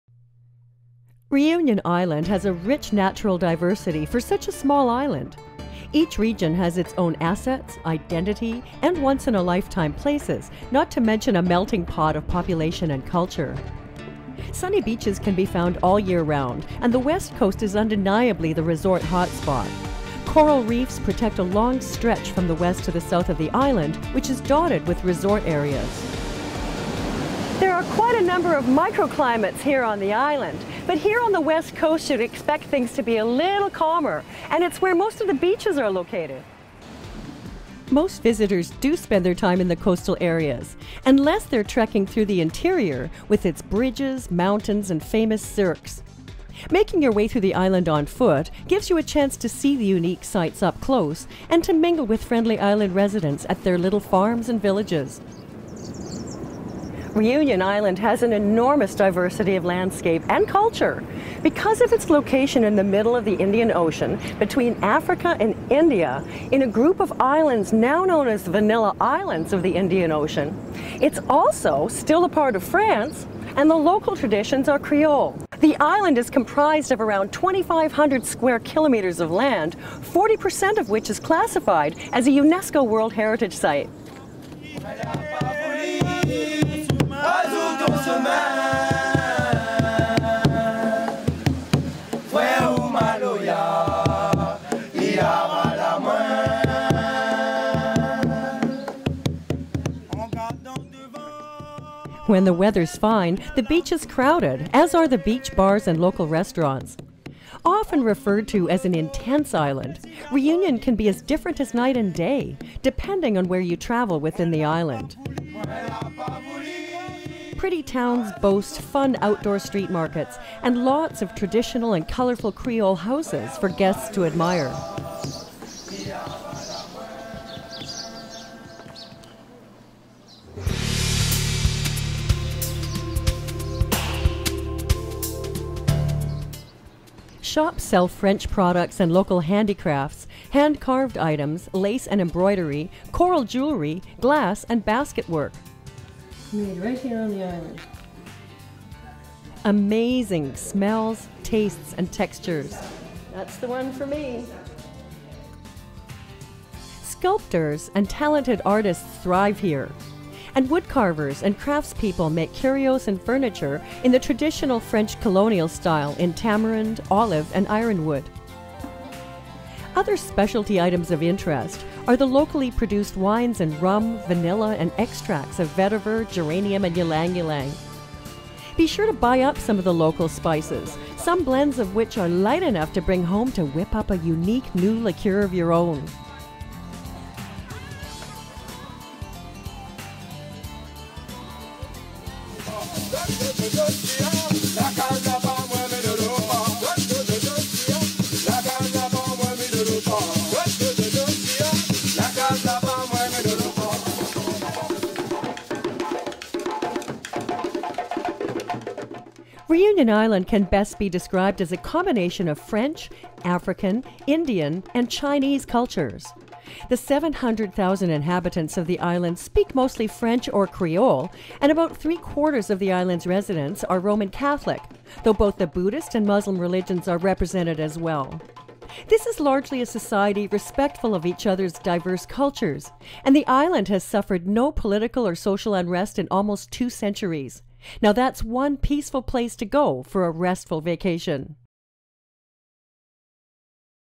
Reunion Island in the Indian Ocean is a French-Styled Paradise
Located in an area known as The Vanilla Islands of the Indian Ocean, Réunion is one of the overseas departments of France. Like the other overseas departments, Réunion is also one of the 27 regions of France (being an overseas region) and an integral part of the Republic with the same status as those situated on the European mainland. Travel TV producer Bea Broda travels to some  of the towns and beaches that help make the island a popular vacation retreat.